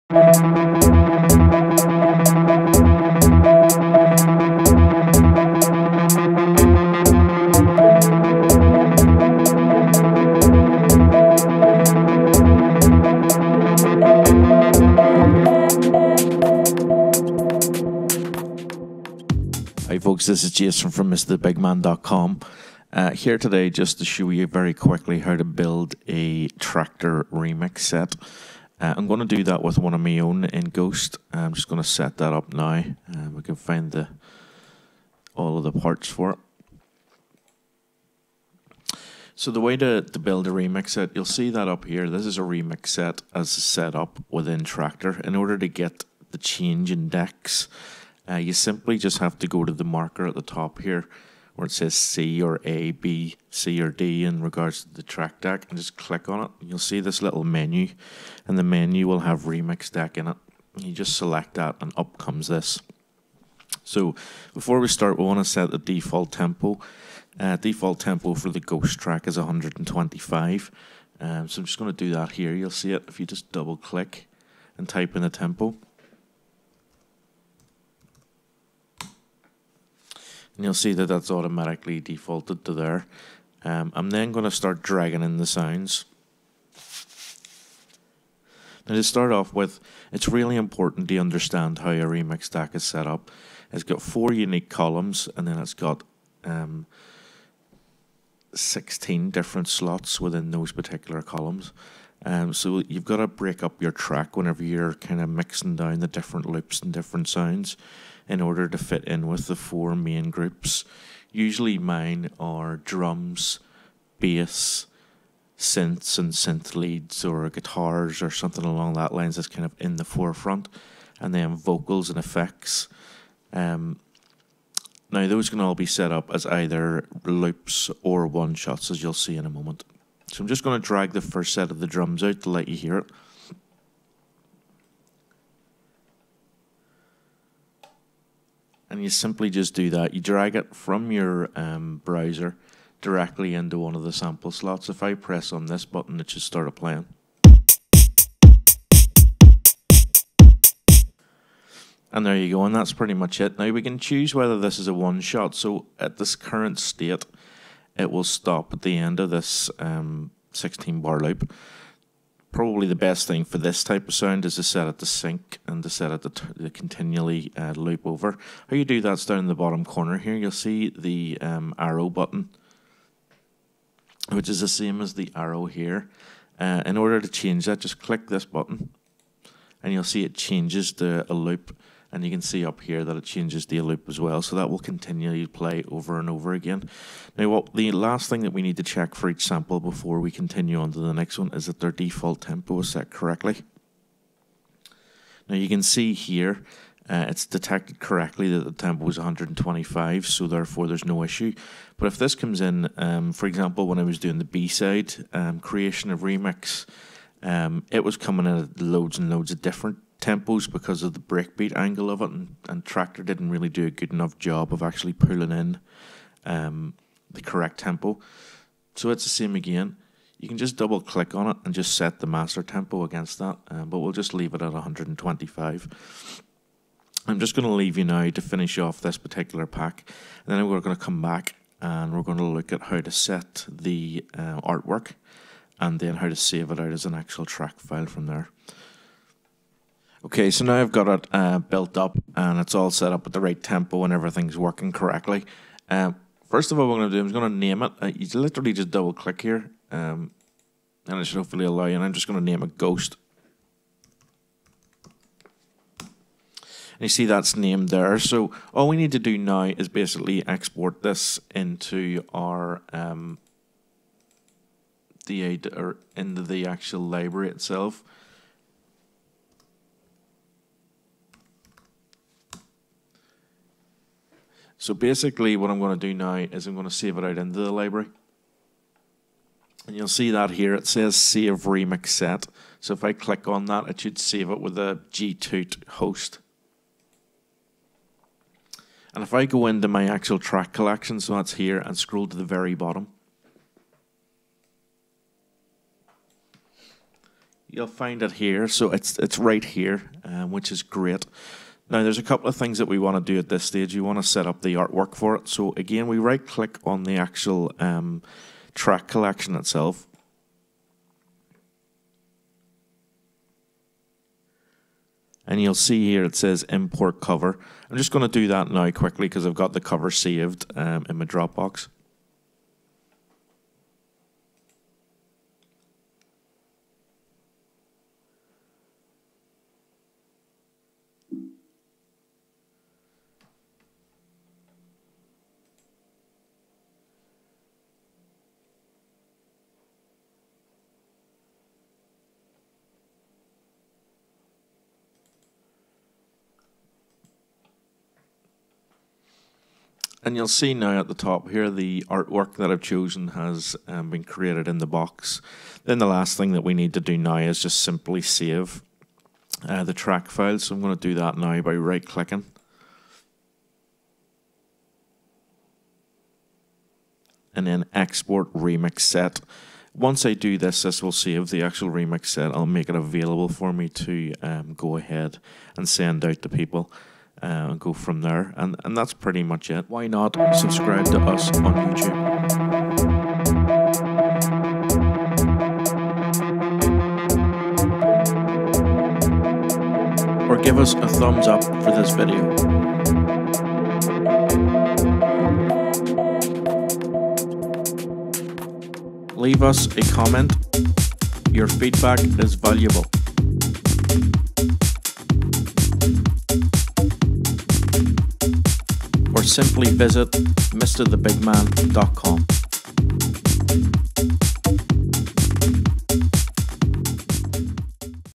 Creating Traktor Remix Sets
mrthebigman shows you how to create a Traktor Remix Set from scratch.